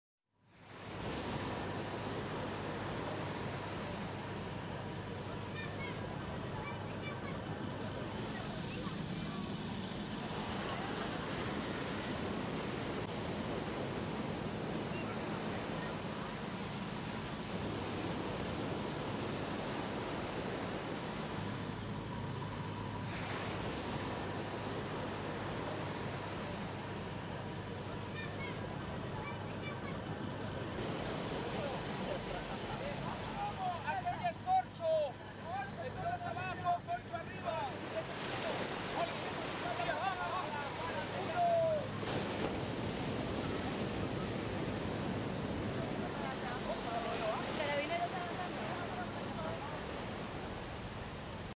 whale
Giant Blue Whale
Giant Beached Blue Whale Saved by fishermen Off Chille Coast